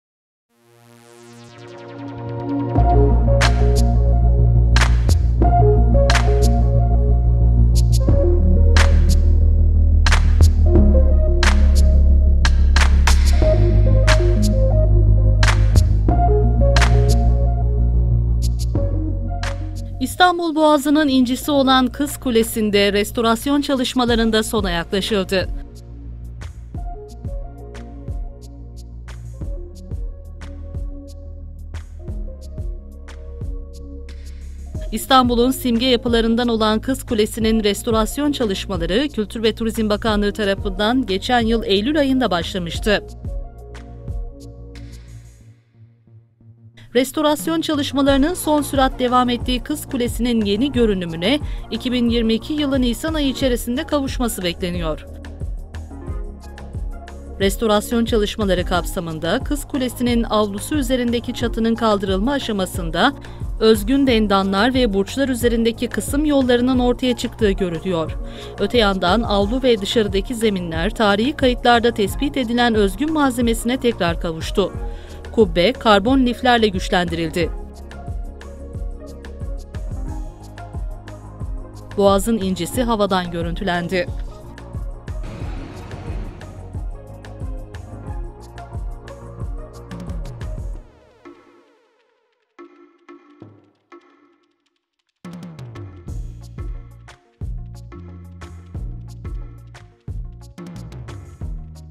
Restorasyon çalışmaları son sürat devam eden Kız Kulesi havadan görüntülendi
İstanbul Boğazı’nın incisi olan Kız Kulesi’nde restorasyon çalışmalarında sona yaklaşıldı. Kültür ve Turizm Bakanı Mehmet Nuri Ersoy’un 2022 yılının Nisan ayında tamamlamayı hedeflediklerini belirttiği Kız Kulesi’ndeki restorasyon çalışmalarının son hali havadan görüntülendi.